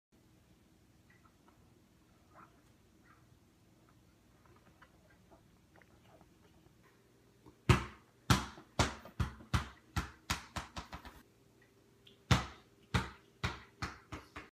Vetra Volleyball Soft Touch Volley Ball Official Size 5 Yellow /Blue/ White
This Vetra Volleyball is well made, nice and durable. It was easy to pump it with no problems. This ball is so soft and perfect for playing volleyball with young kids that don't want to hurt their arms with every hit. My kids really love it, I would recommend this product.

*I received this product free of charge in exchange for an honest and unbiased review.